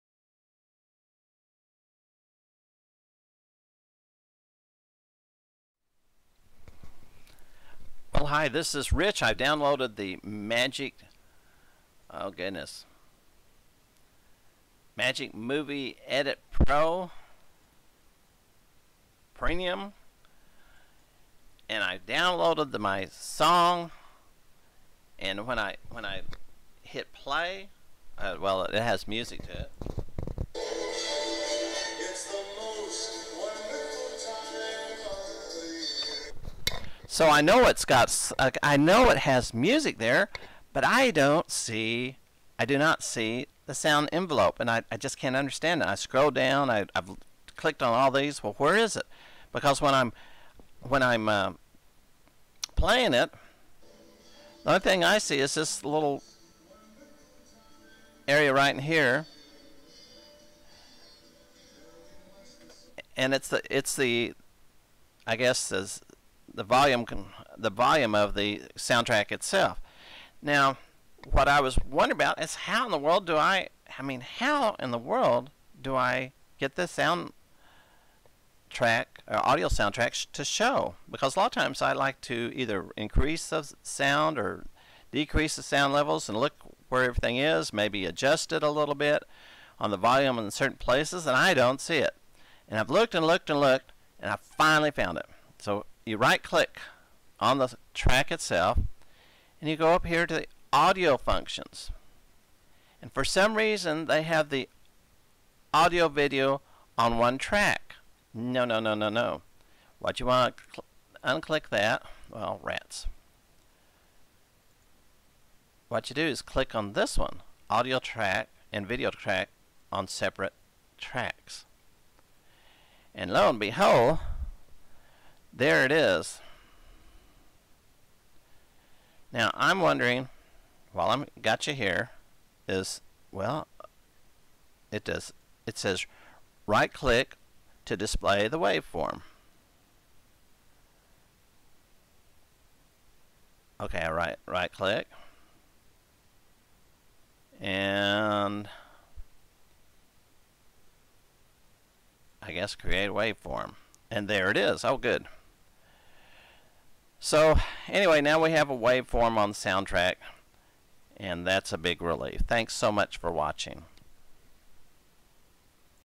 Where is the Audio Soundtrack MAGIX Movie Edit Pro Premium
Where is the Audio Soundtrack on Magix Movie Edit Pro Premium?  To me it was so very hard to find!!!